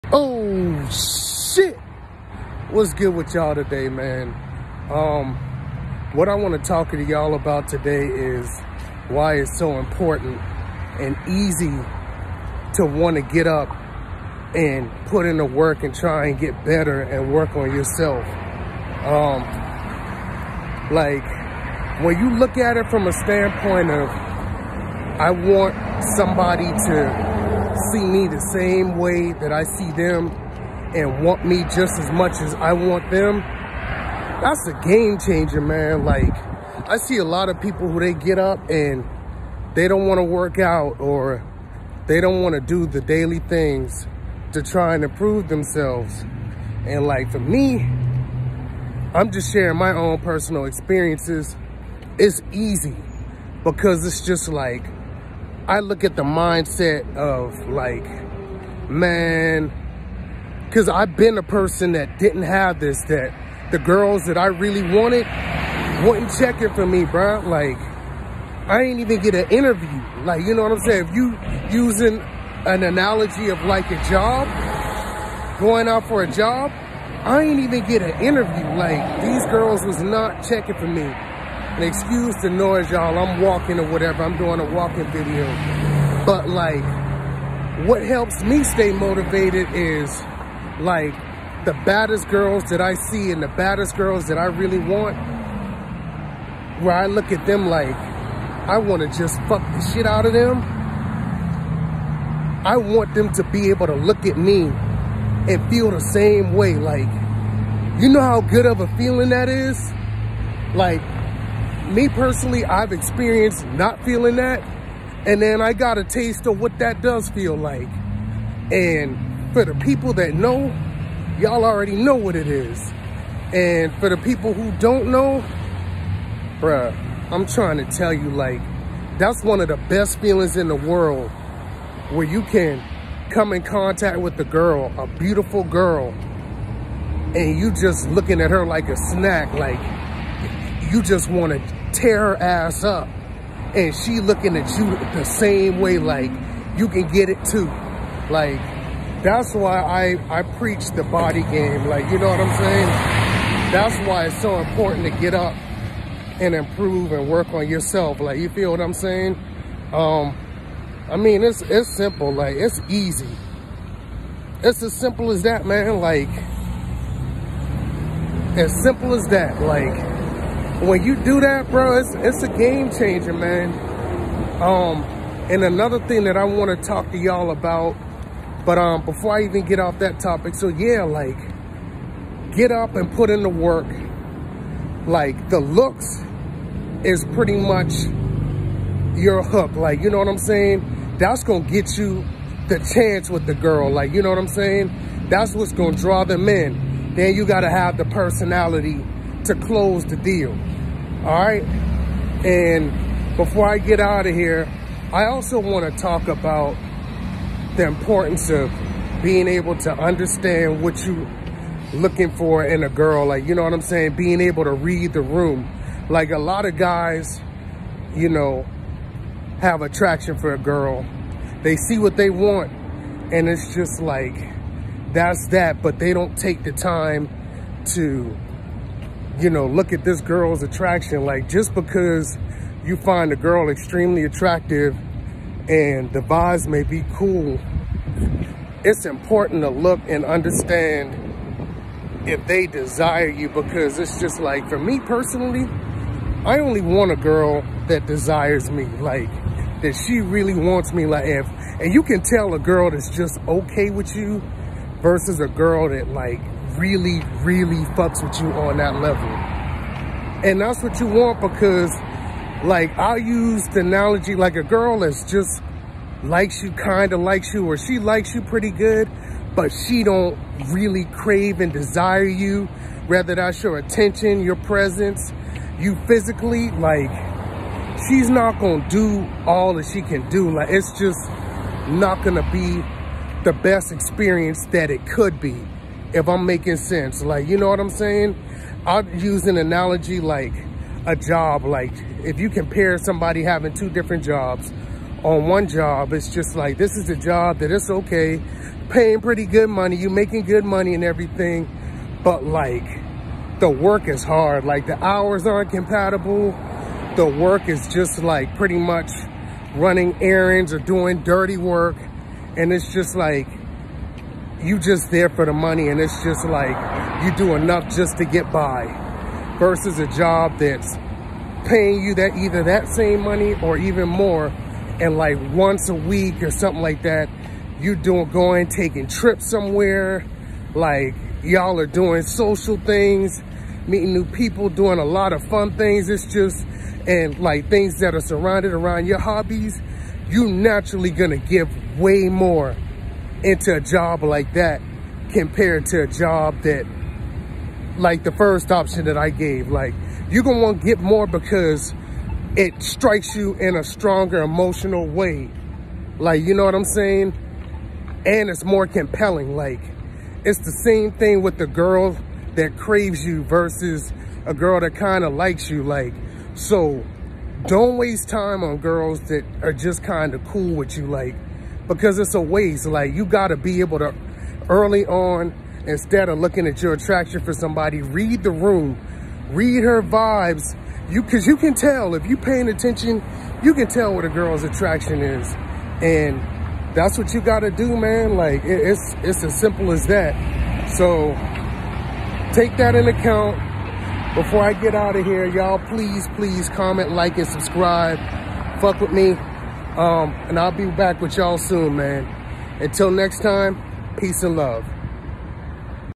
Why it’s SO important to Grind & Improve 💯👌🏾
When YOU get better, your Life & options get better. Quality of dating choices, finances, health. everything goes up. And Who doesn’t want that. Check the video for the breakdown.